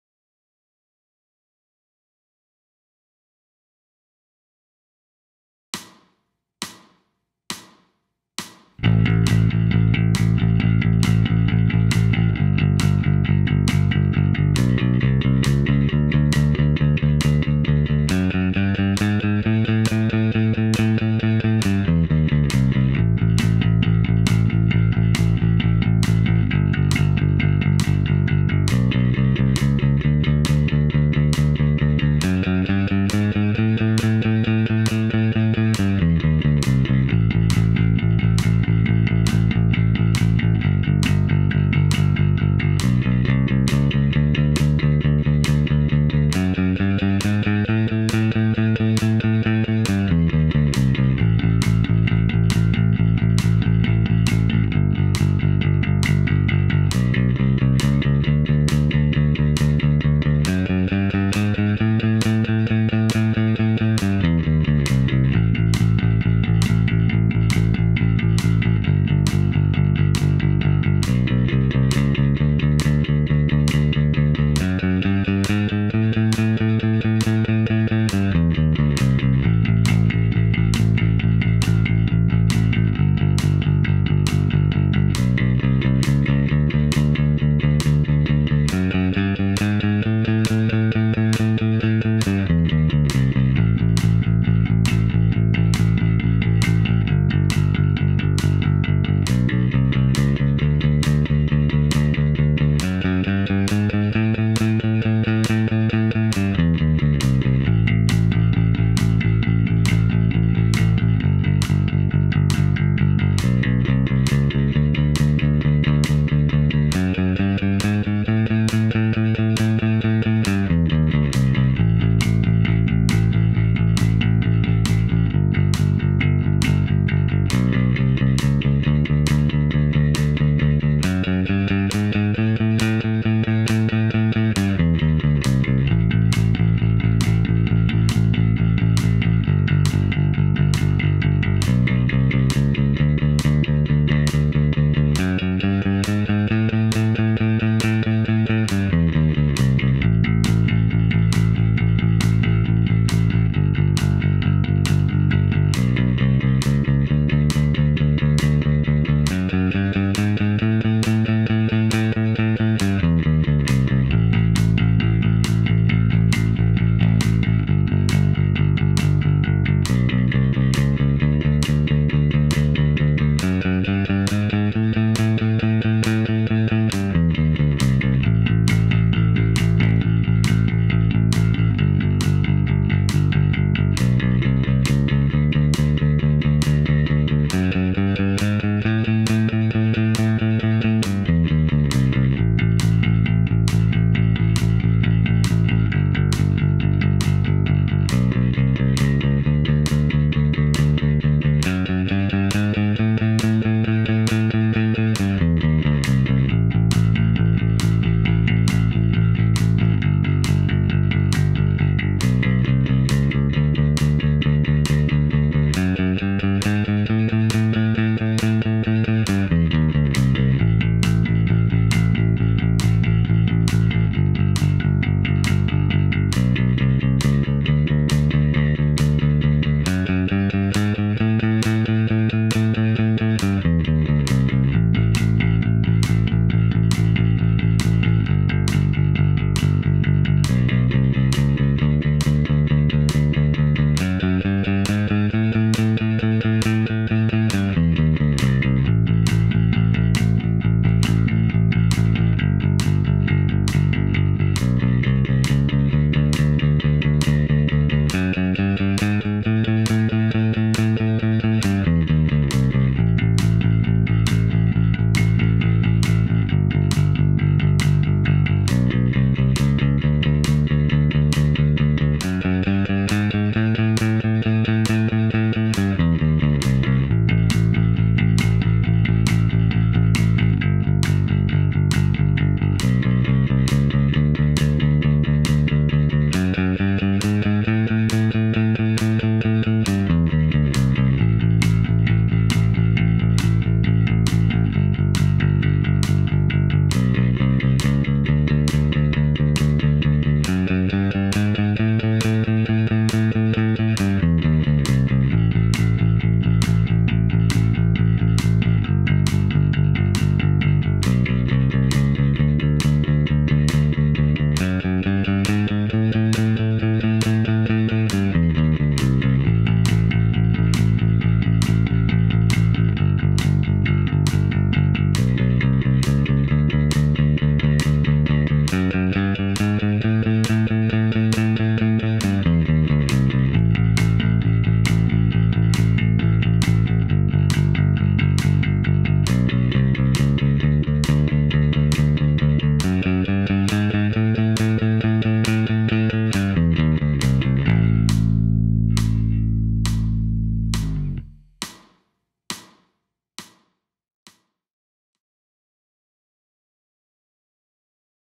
[BPM=136] 5 minutes of endurance down picking practice for bass guitar
Bass : Moon JJ-4
Gear : TECH21 SANSAMP Bass Driver, Roland Quad-Capture, Cubase Pro
Tuning :Normal
Date : 2023/10/01